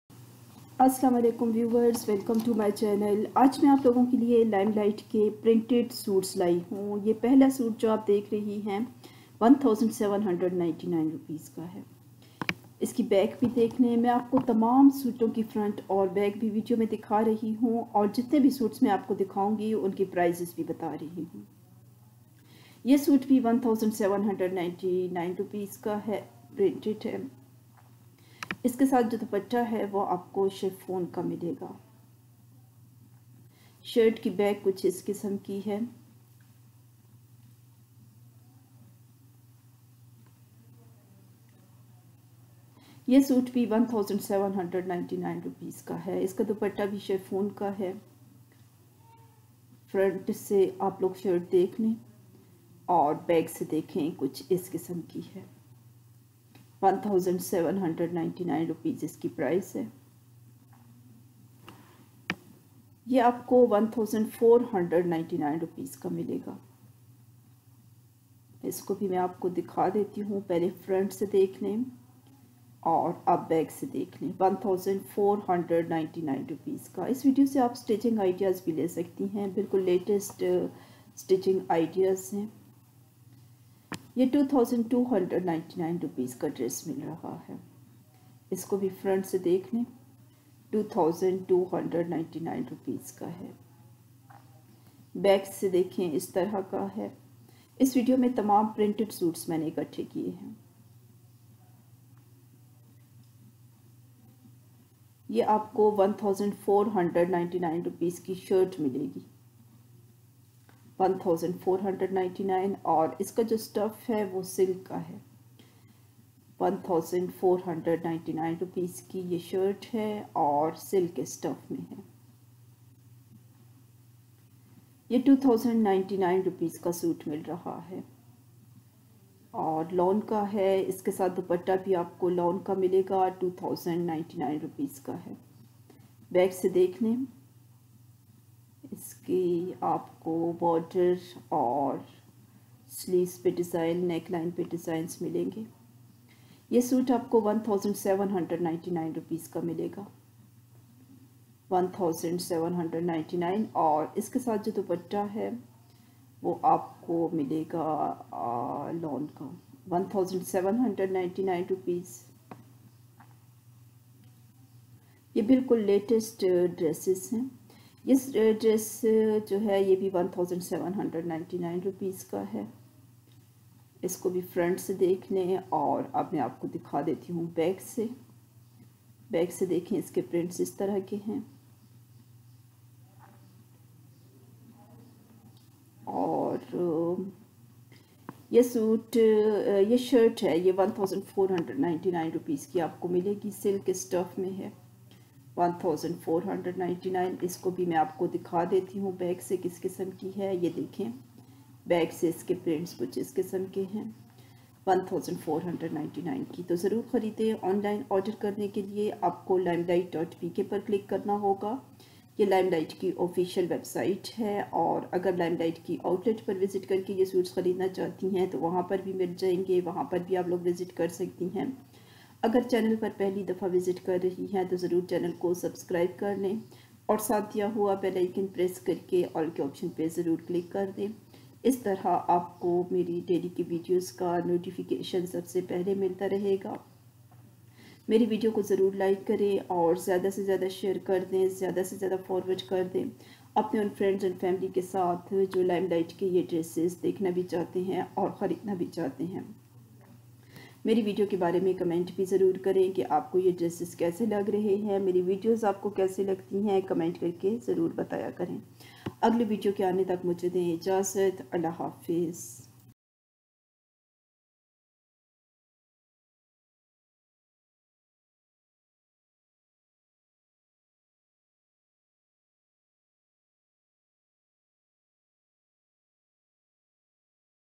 LIMELIGHT NEW AND LATEST PRINTED SUITS WITH LATEST TRENDY FASHION 2021//LIMELIGHT//FASHION & FASHION
IN THIS VIEO YOU CAN SEE LIMELIGHT DRESSES.THESE ARE UNSTITCHED DRESSES.PRICES OF THESE DRESSES ARE MENTIONED IN THE VIDEO.